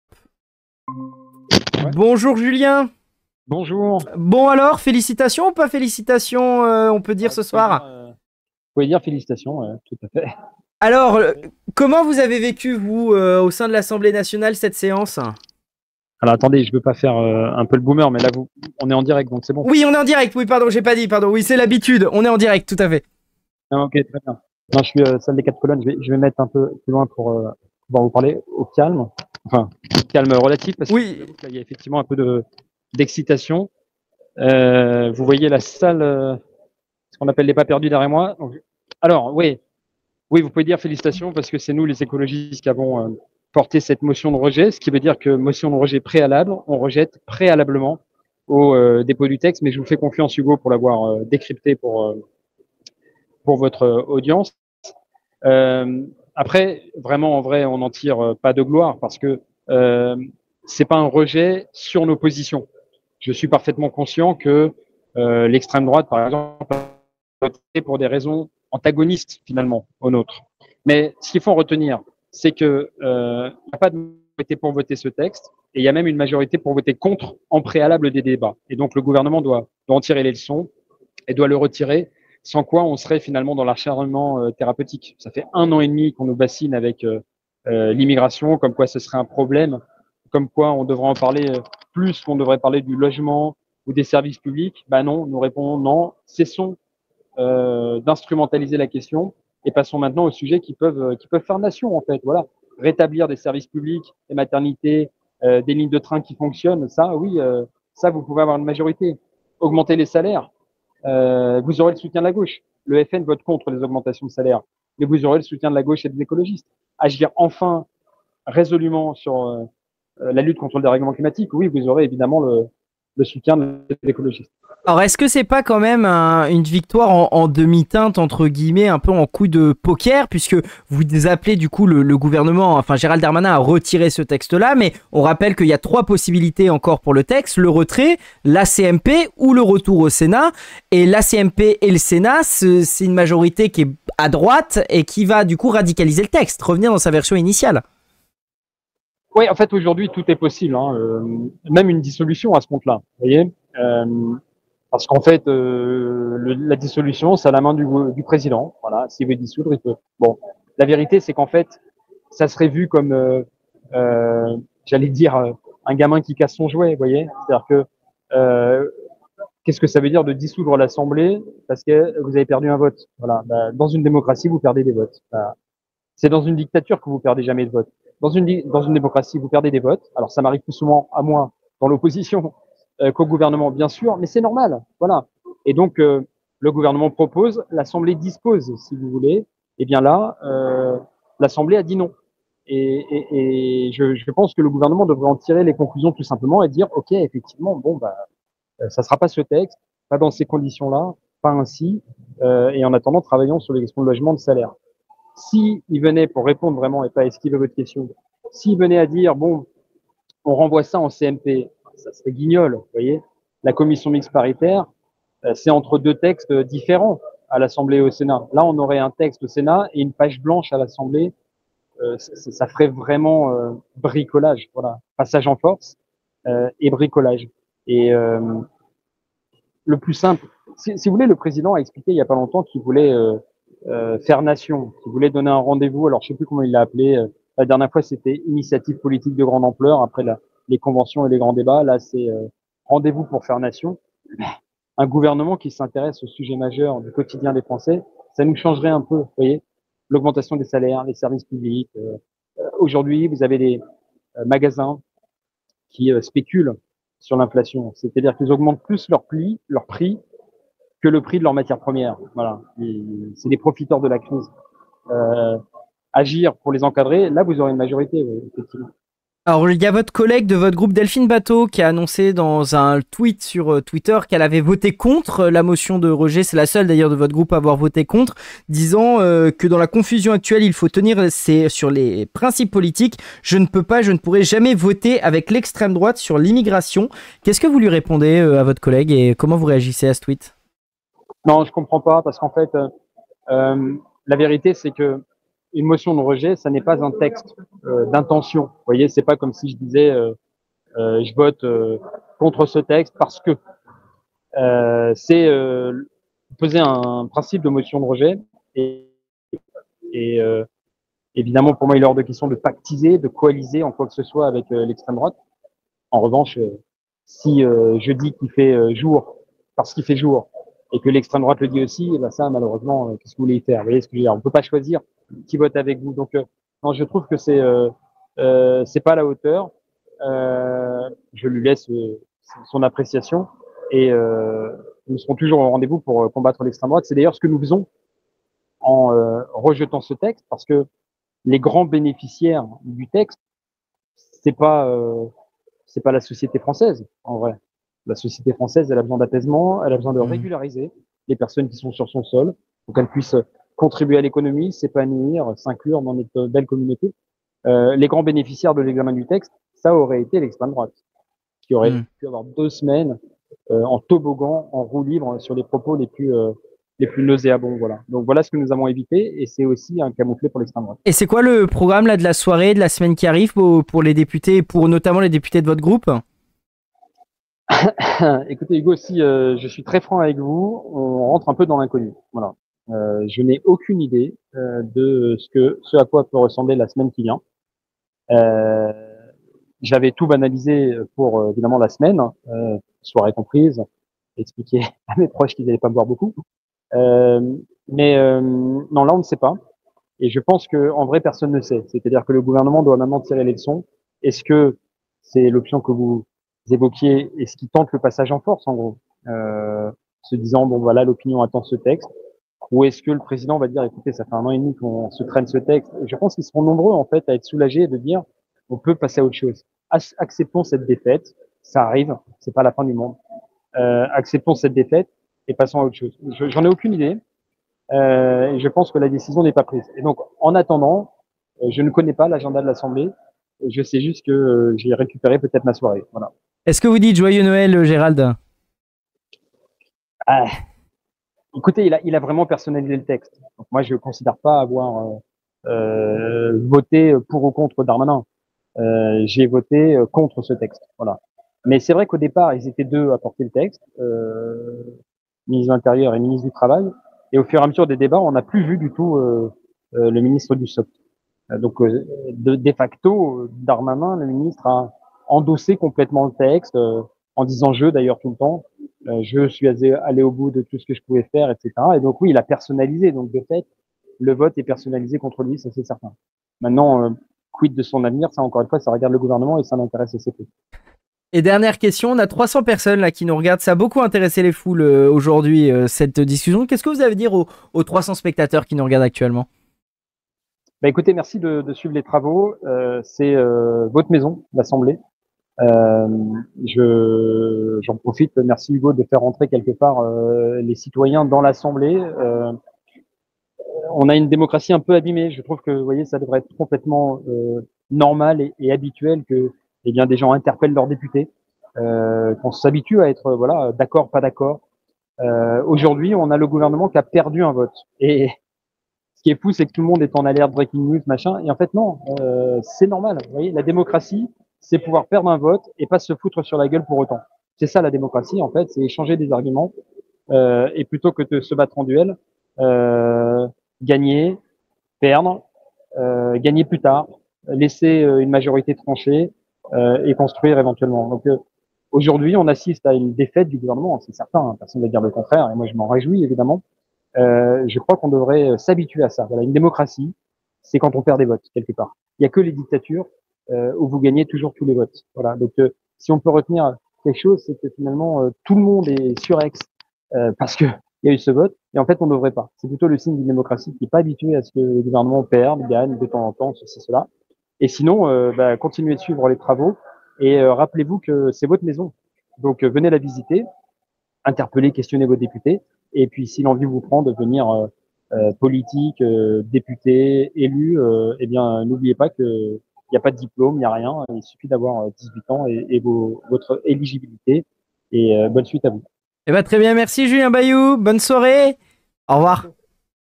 INTERVIEW : Le député Écologiste Julien Bayou réagit au rejet de la loi immigration !
Interview du député Écologiste Julien Bayou , pour parler de l'adoption de la motion de rejet de la loi immigration en première lecture de l'AN.